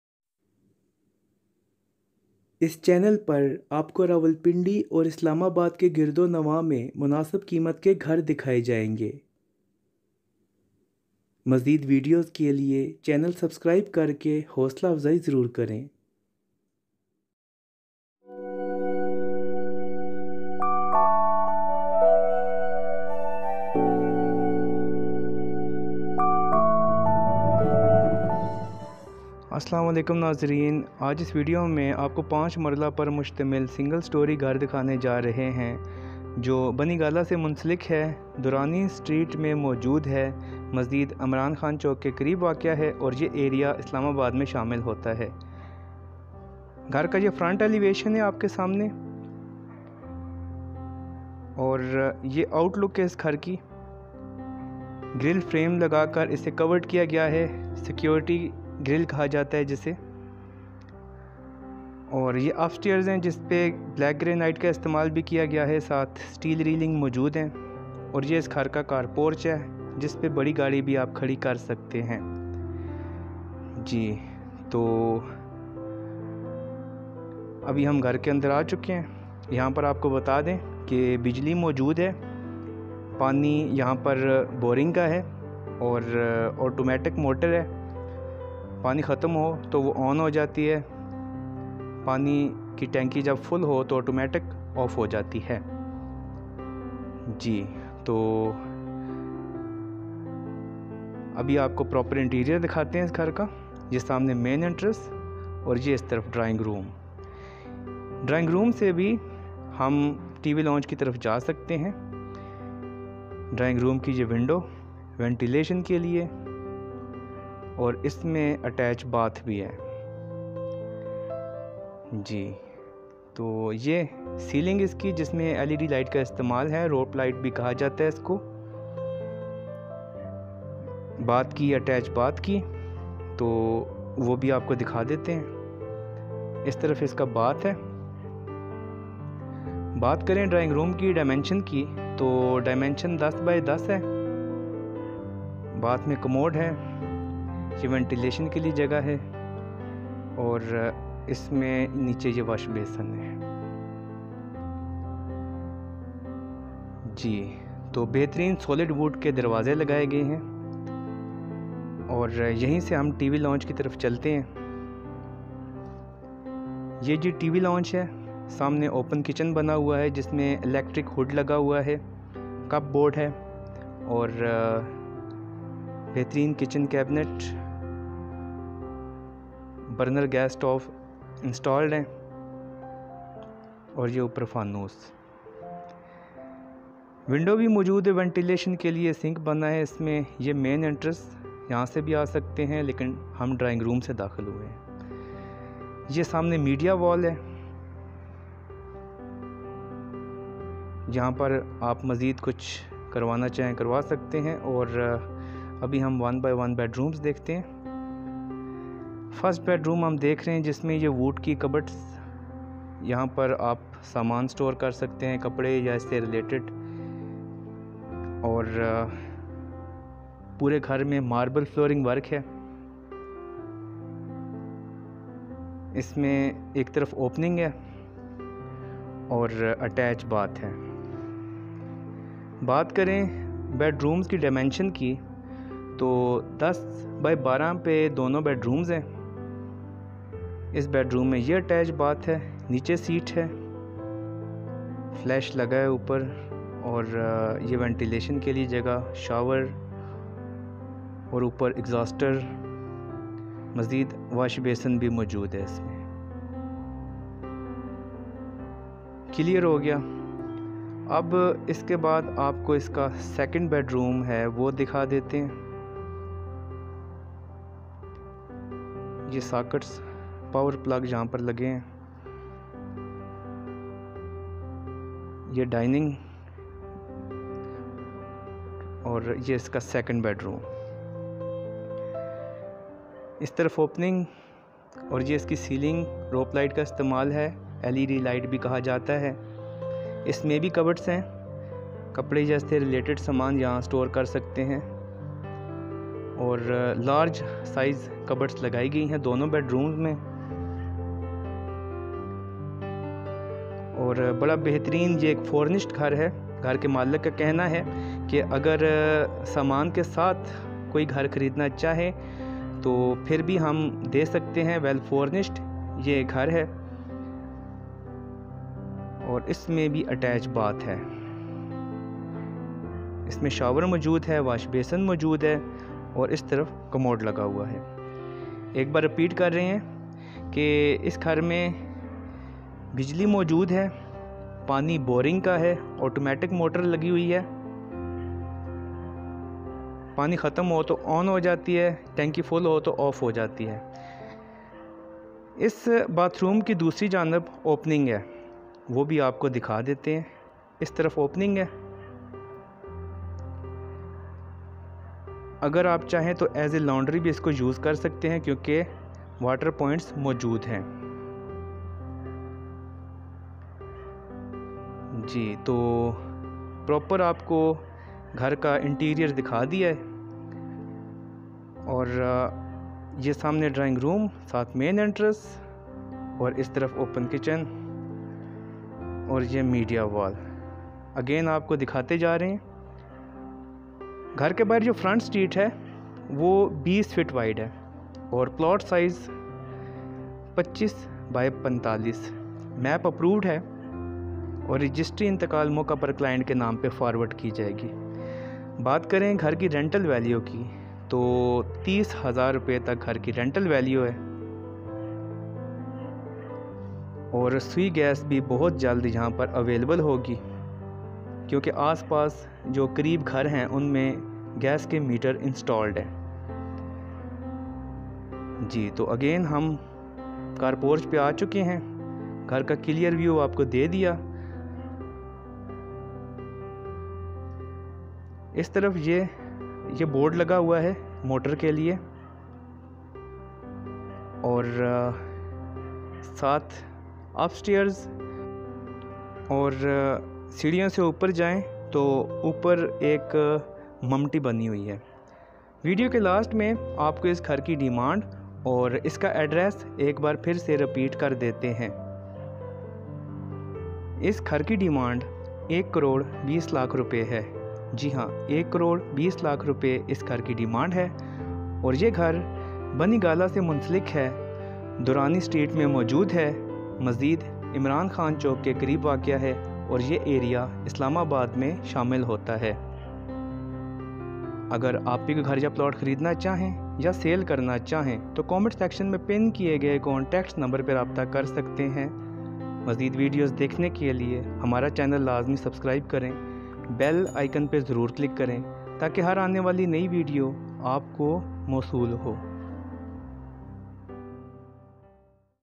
House No.559 | House in Bani gala | Islamabad
In this video we are going to show you 5 marla new house in Durani street and this house located in Bani Gala near Imran Khan Chowk and this area connected to Islamabad. Electricity,water available here and this is a newly constructed furnished house. For more information watch this video and give us your feedback. @mohsinproperty